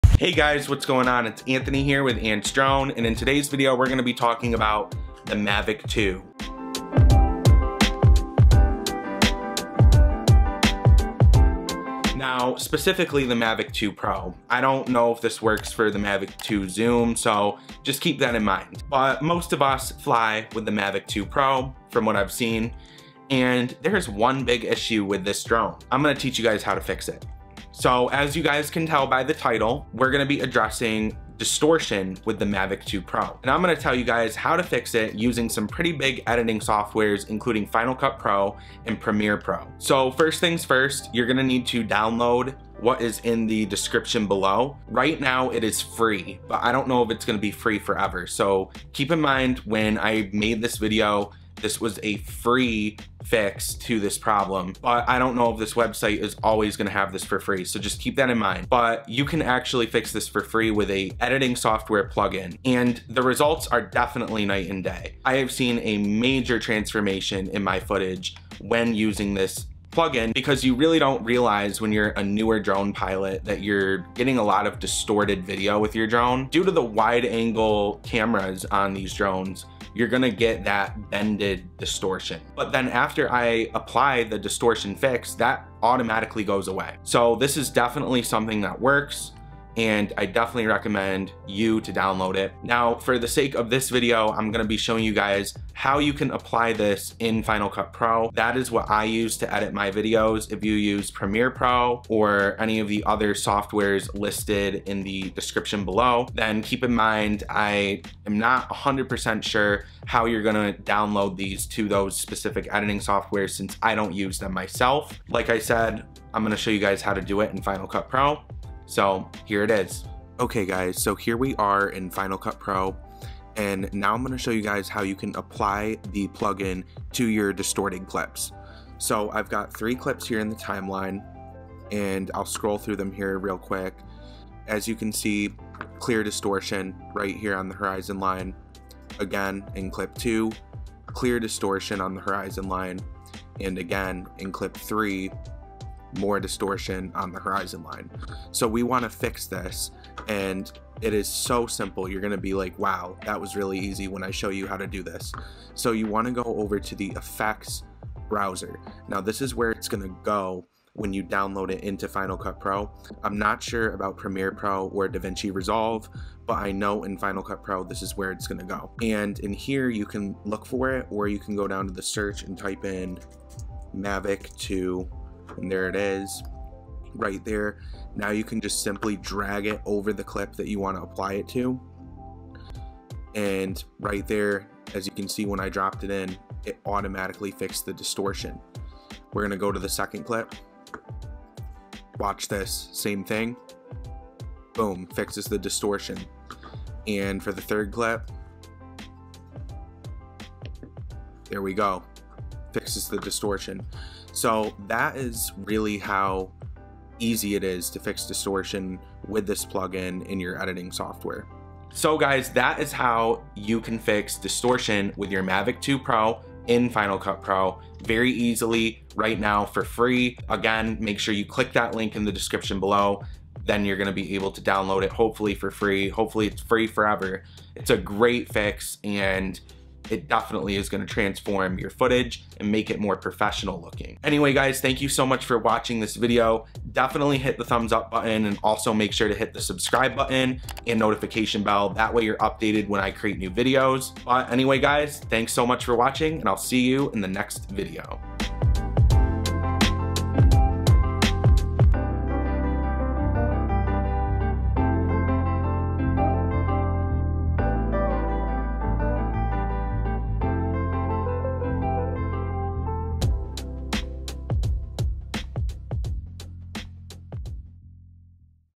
DJI Mavic 2 Pro Distorted Video Fix | Final Cut Pro X, Premiere Pro and Davinci Resolve
Learn how to easily fix distortion coming from the camera on the DJI Mavic 2 Pro for free with Final Cut Pro X, Adobe Premiere Pro and Davinci Resolve.

If you are new to the channel my name is Anthony. It helps my video rank higher in the algorithm.